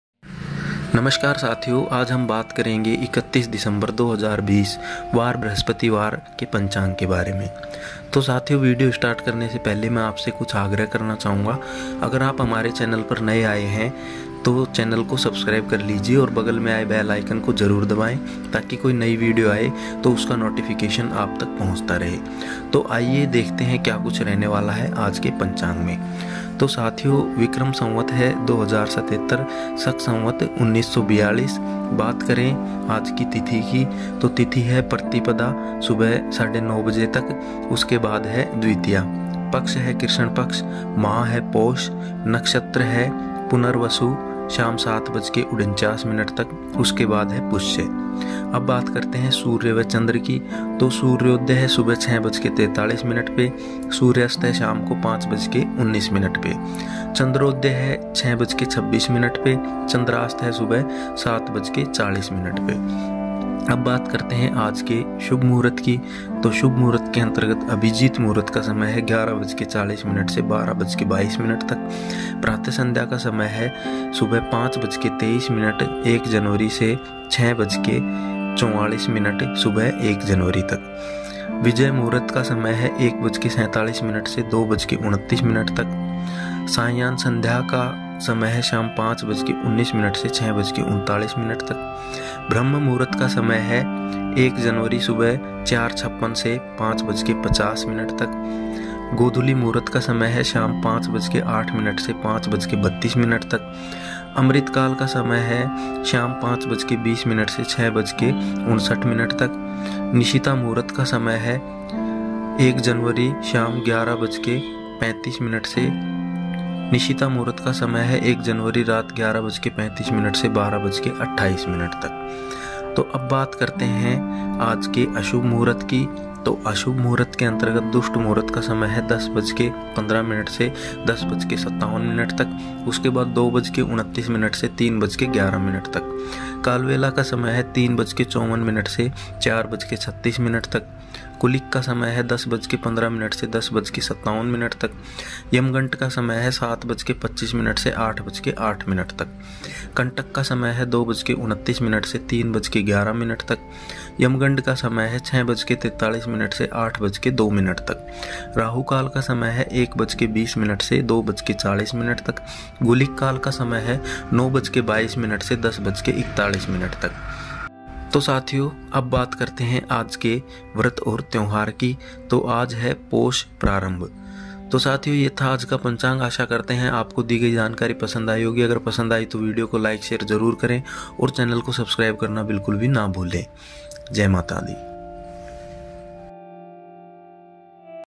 Aaj ka Panchang 31 December 2020 | आज का पंचांग | शुभ मुहूर्त , राहुकाल | Thursday Last Panchang 020
किसी भी शुभ कार्य को करने के लिए सही मुहूर्त, सही समय, और सही नक्षत्र का होना जरुरी होता है, आज हम आपको आज के दिन का सही समय जो की सही कार्य करने का है, और आज कब कौन सी दशा कौन से टाइम में चल रही है उसका समुचित विवरण हिन्दू कैलेंडर और हिन्दू पंचांग के अनुसार दे रहे है, आज आपका दिन मंगलमयी रहे, यही शुभकामना है। आज का दिन शुभ कार्य के लिए सही कार्य है या नहीं आज के चौघड़िया के अनुसार जानें।